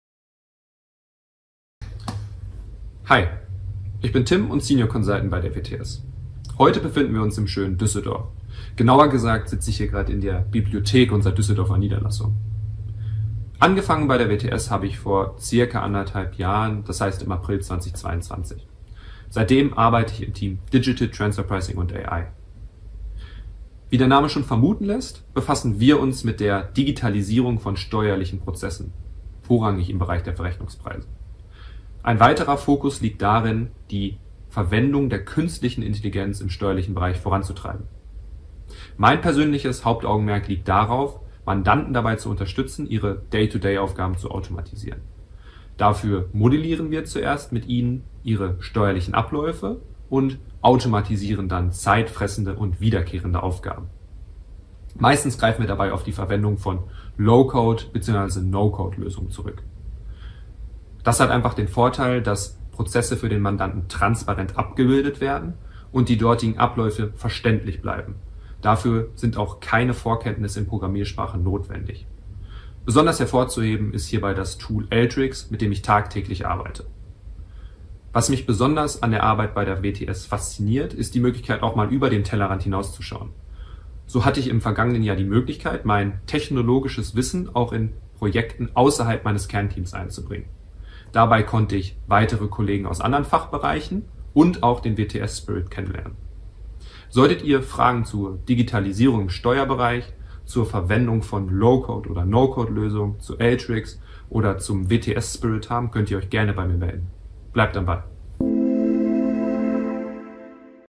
pioneeroftoday Tim Strotmann
Wie kann man wiederkehrende Alltagsaufgaben im Steuerbereich durch den Einsatz künstlicher Intelligenz erleichtern? 🤖 Das erfährst Du von unserem pioneeroftoday Tim. Er arbeitet seit Anfang 2022 bei WTS im Bereich Digital Transfer Pricing & AI und sieht einen klaren Vorteil im Einsatz von KI-basierten Tools: Transparenz für alle Beteiligten, unabhängig von Programmierkenntnissen.

Klingt spannend? Dann schau Dir das Video an und bleib am Ball! 🔴